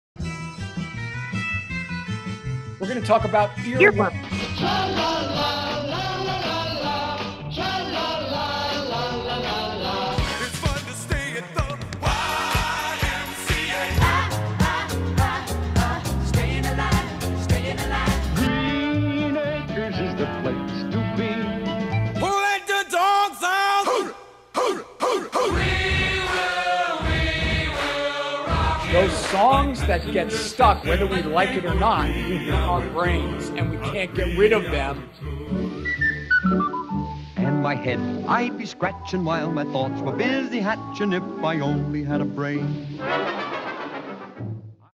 Ear Worms Promo
On the next episode of Romick Brothers we talk about Ear Worms, those songs that stuck in our heads for no apparent reason, why it happens and how to combat it.